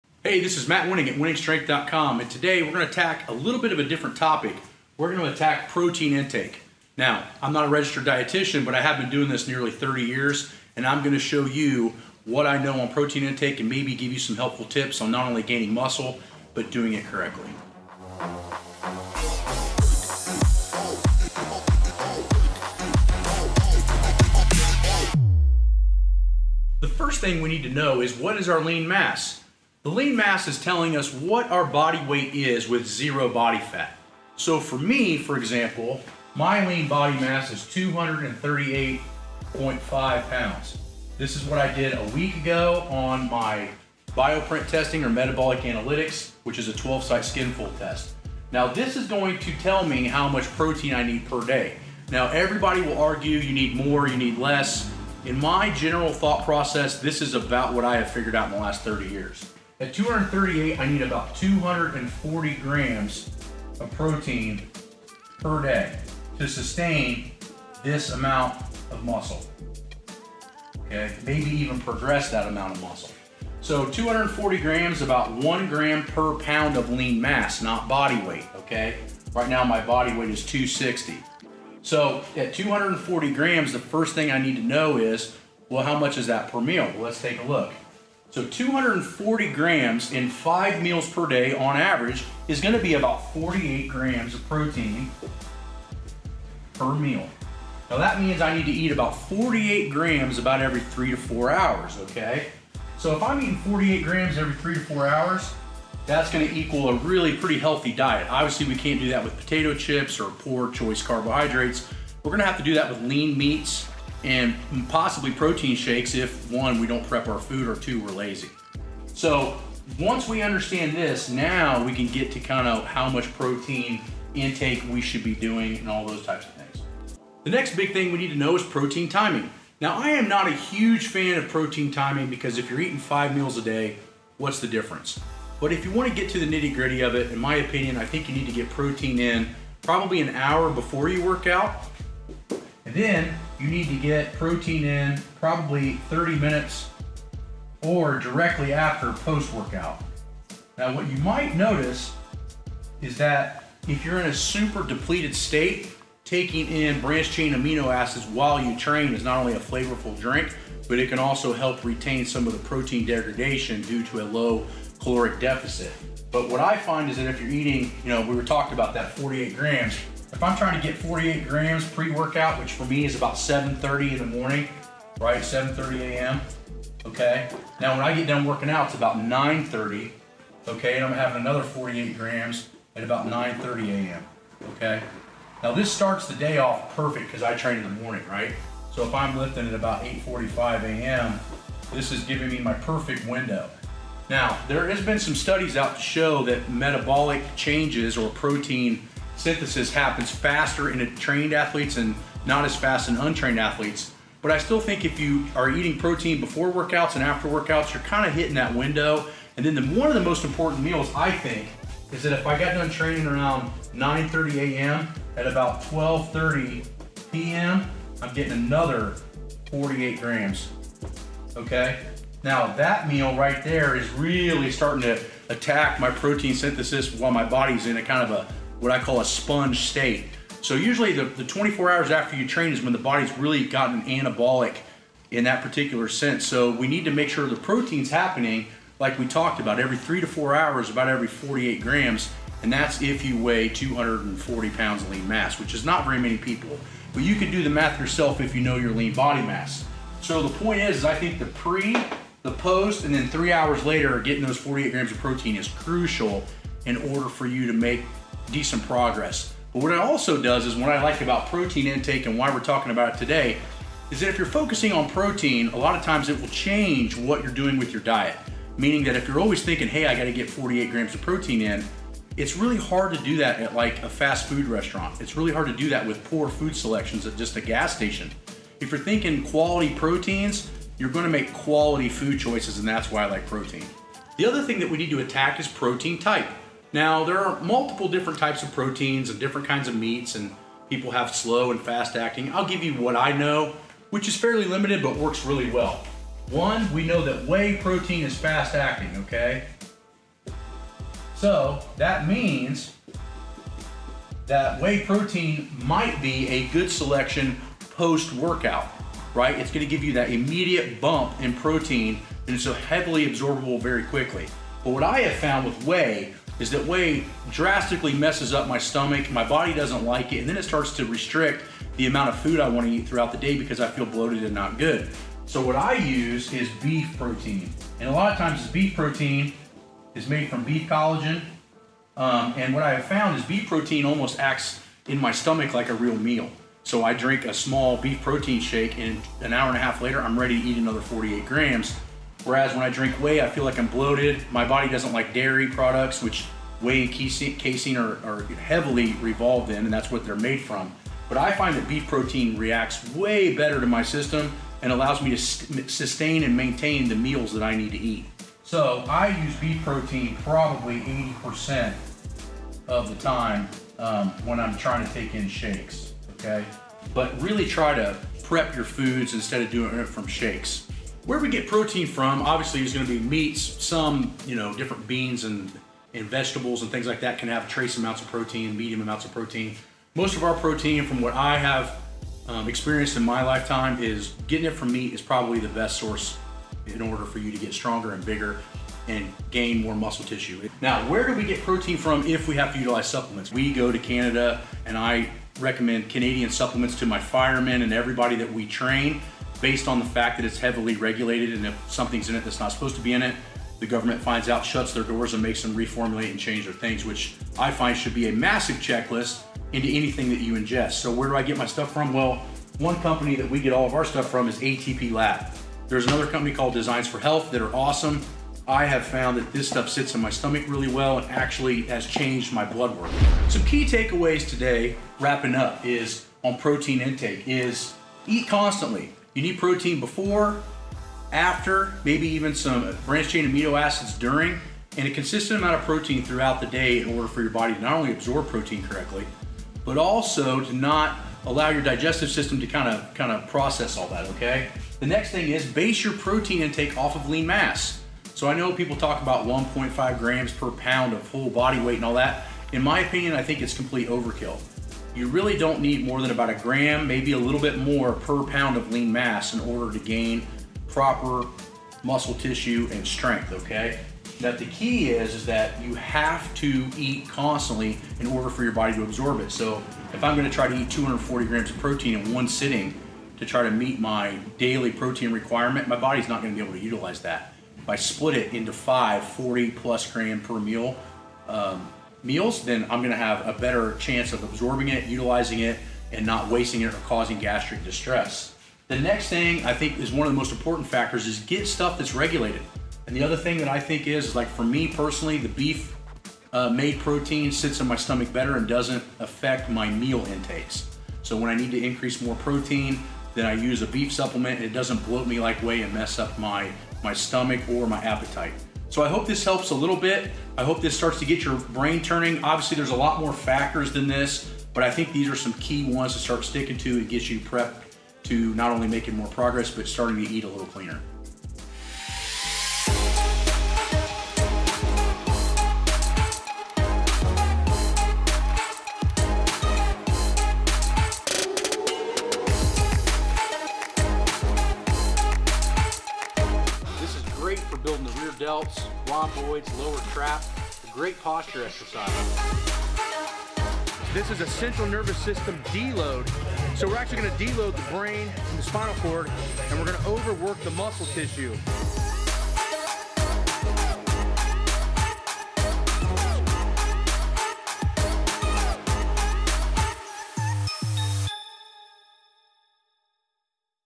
PROTEIN INTAKE FROM SOMEONE WITH 30 YEARS OF EXPERIENCE (How I keep over 240 lbs lean mass)
Matt Wenning goes over what he's learned from experience about protein intake from his 30 years of experience and how he is able to maintain such a large amount of lean mass.

Matt has helped feed some other strongmen too!

Want to take your Powerlifting seriously? Grab the Powerlifting Training Manual to get step by step training.

Now you can pick up a Percussion Massage Gun for more effective recovery days and back pain!